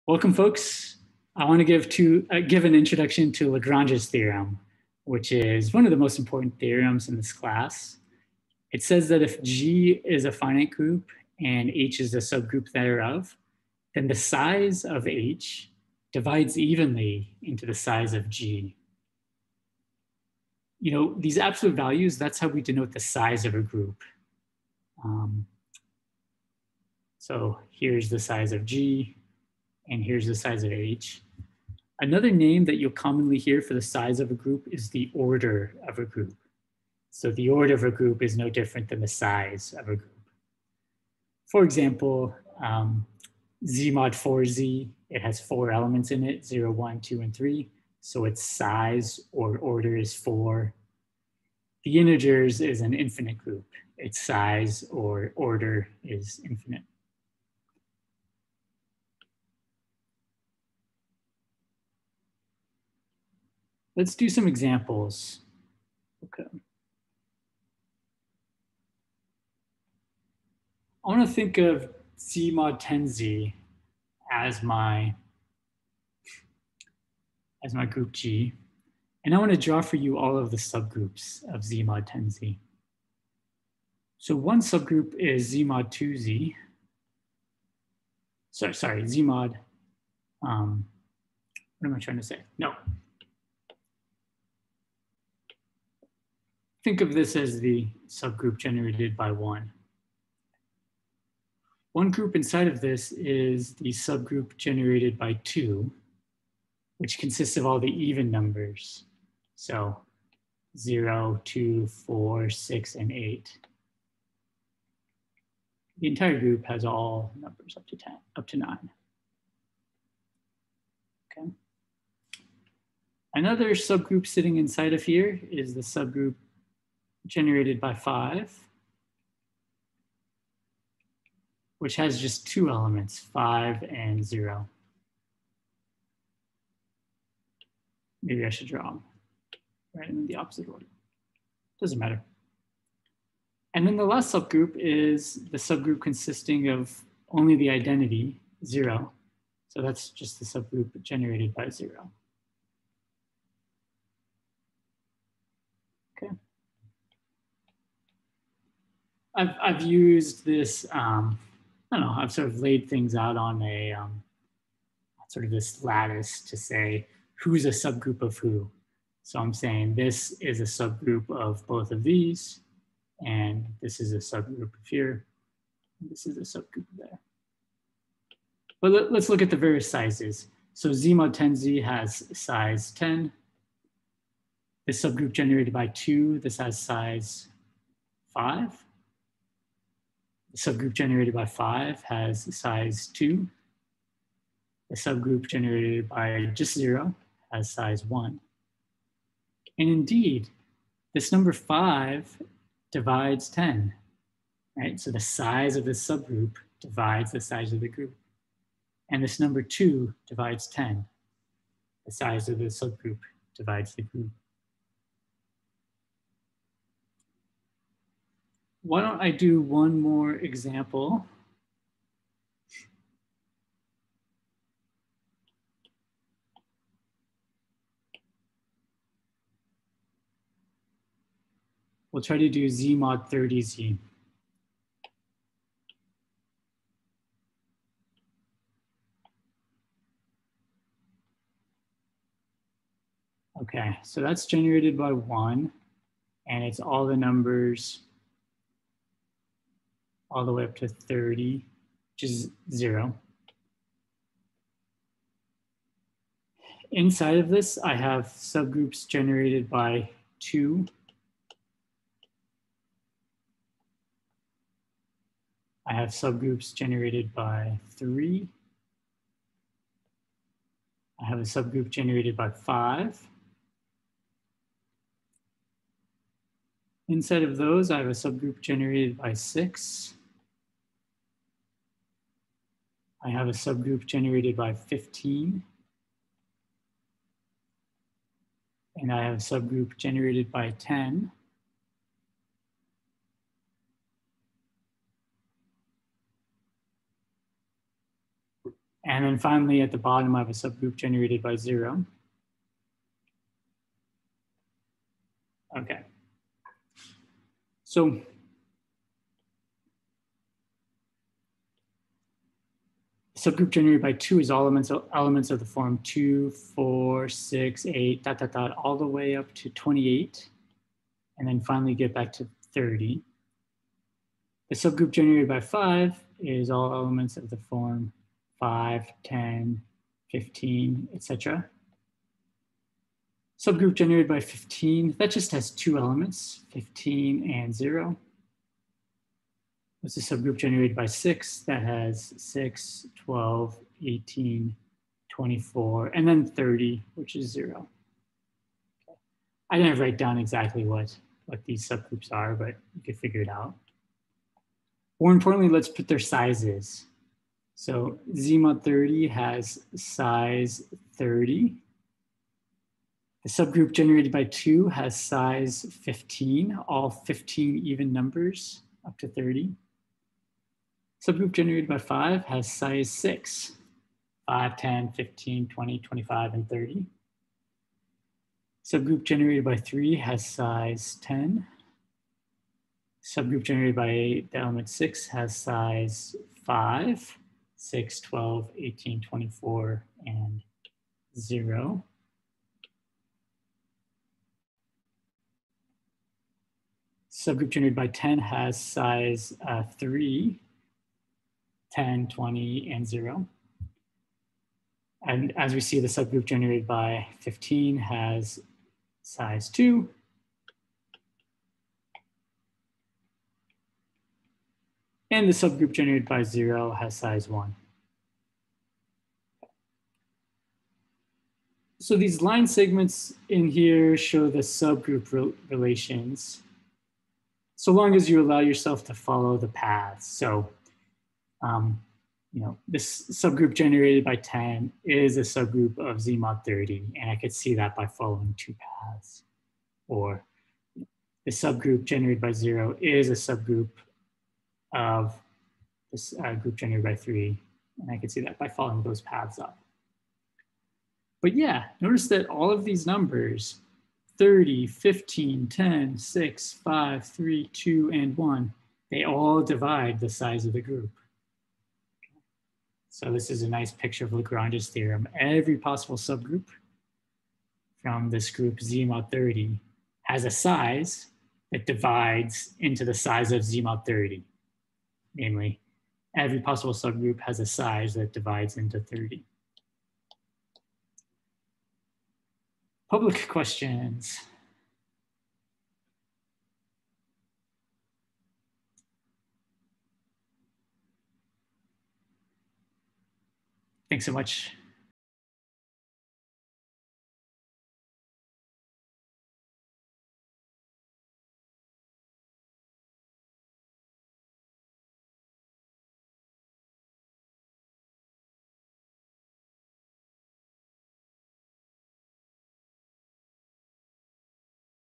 Abstract Algebra 35: Lagrange's theorem: a subgroup's size divides evenly into the group's size
Abstract: We explain why you should care about permutation groups. One big reason is that every finite group is isomorphic to some group of permutations. Therefore, any finite group can be cast in the language of permutation groups! In other words, if you understand permutation groups very well, then in some sense you can understand any finite group. We show how to model the symmetries of the square as a permutation group.

This video accompanies the class "Introduction to Abstract Algebra" at Colorado State University: